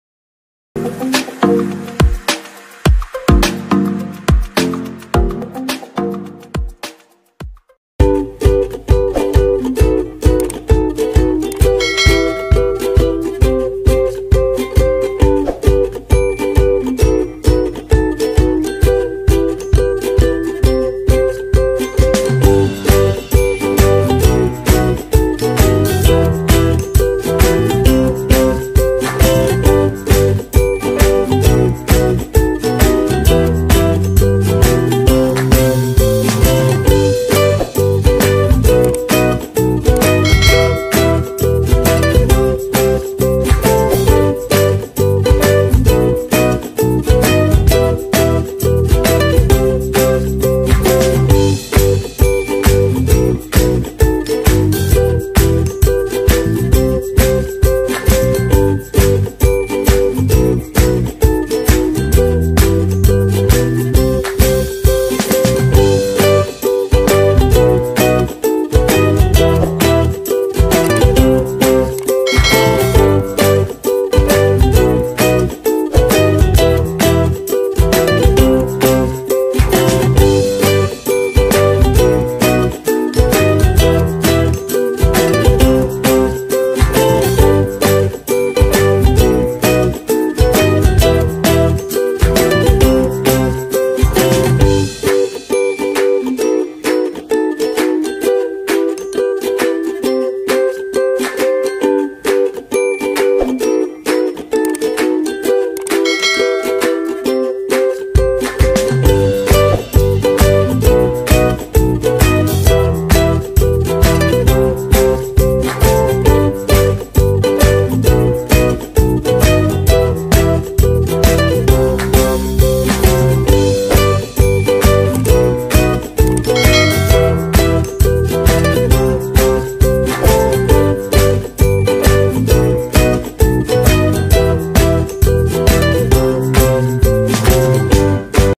Minyak goreng dicampur molto apa jadinya |Tutorial DIY
Lilin aromaterapi bisa dibuat sendiri di rumah,  bahan-bahannya pun mudah didapat.  Yaitu cukup siapkan siapkan sedikit minyak goreng,
Untuk pengharumnya bisa disesuaikan dengan selera.  Bisa molto pewangi atau merk lain.
Untuk lebih jelasnya simak videonya.

TERIMA KASIH